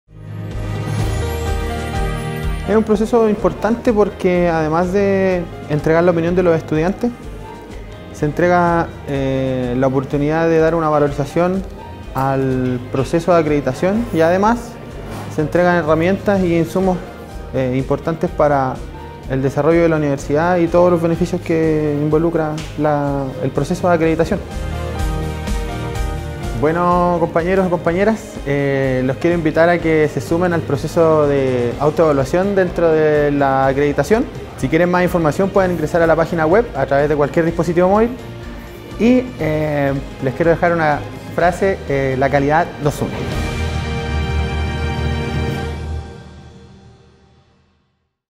Felipe Quintana , trabajador social, egresado UCSH - Acreditación 2020
Los invitamos a sumarse al proceso de acreditación 2020 que está viviendo nuestra Universidad. Recuerda informarte e ingresar a ¡La calidad nos une!